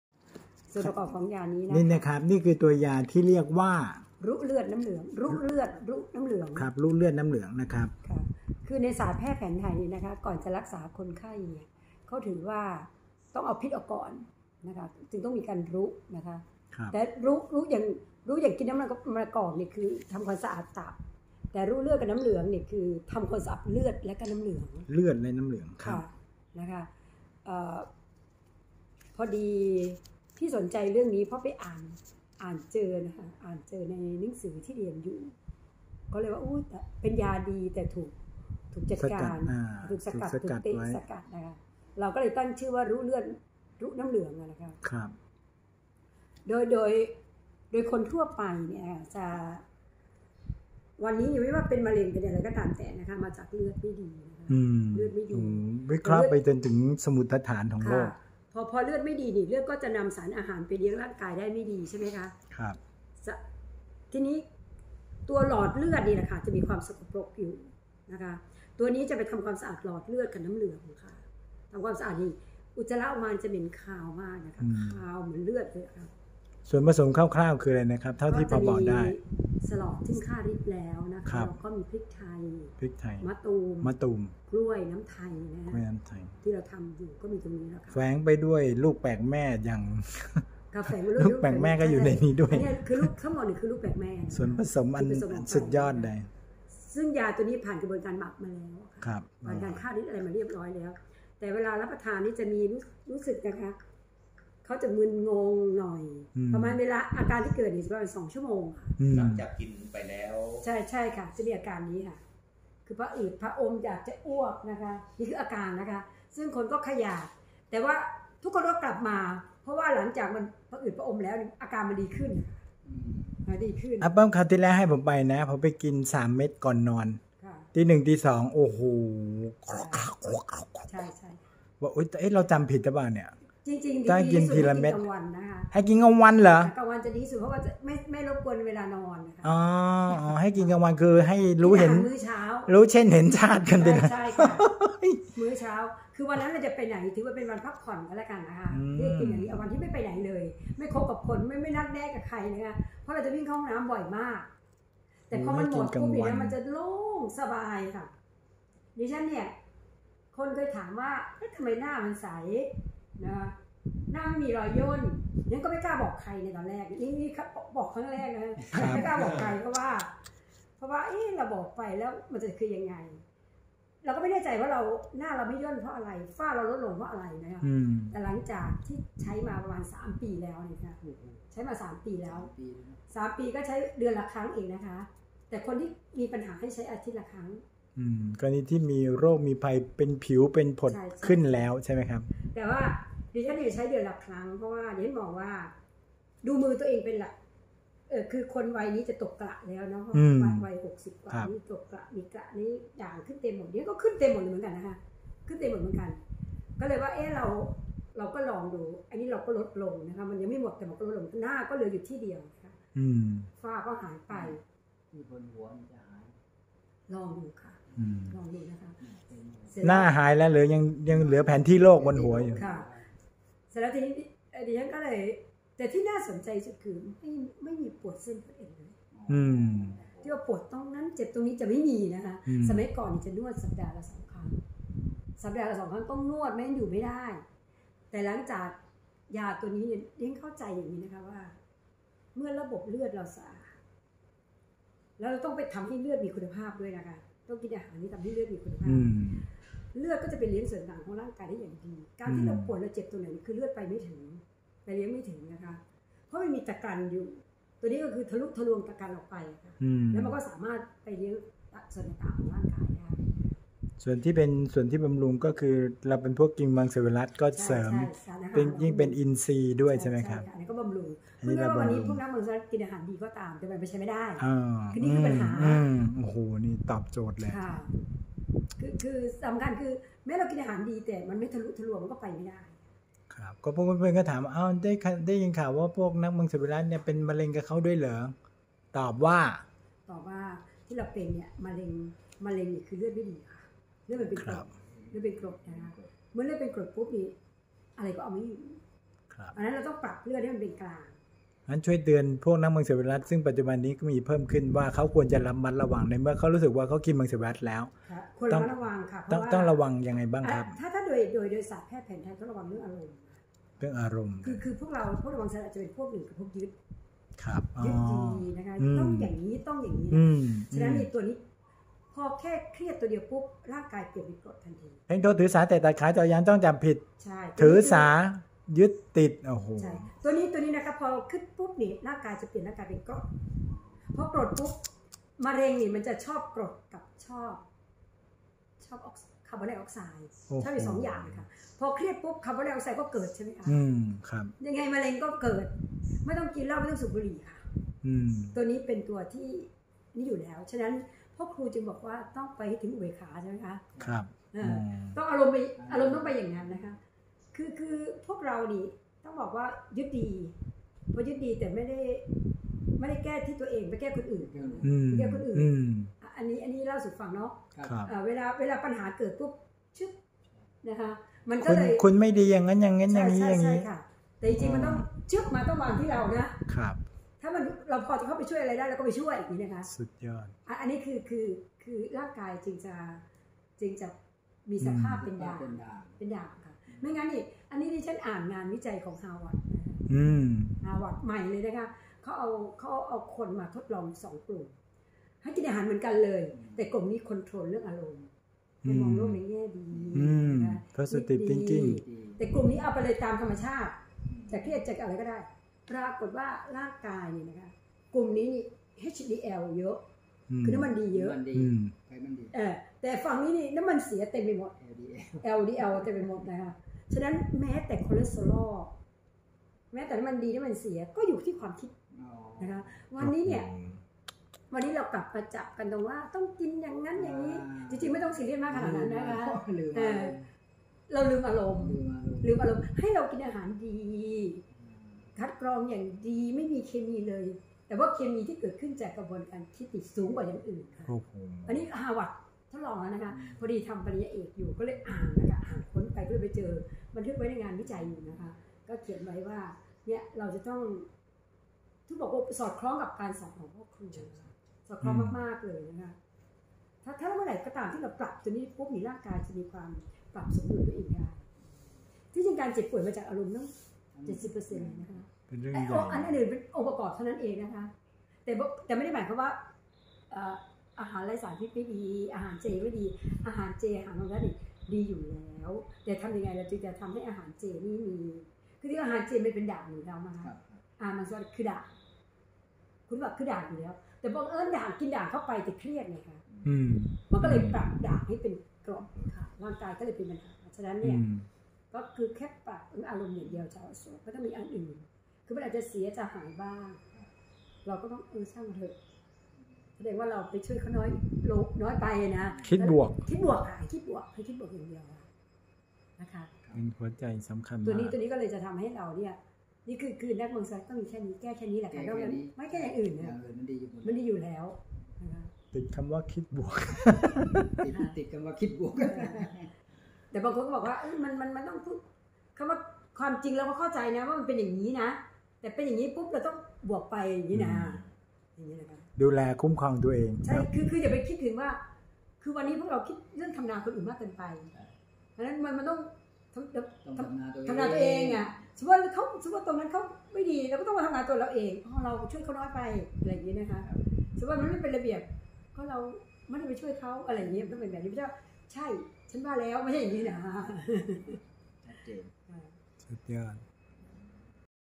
(1)ยารุเลือดรุน้ำเหลือง-สมุนไพรช่วยขับถ่ายอย่างดี(🧡อาเปิ้ม อ.ขวัญดิน สิงห์คำ)
(25 พย. 2563) ณ ศีรษะอโศก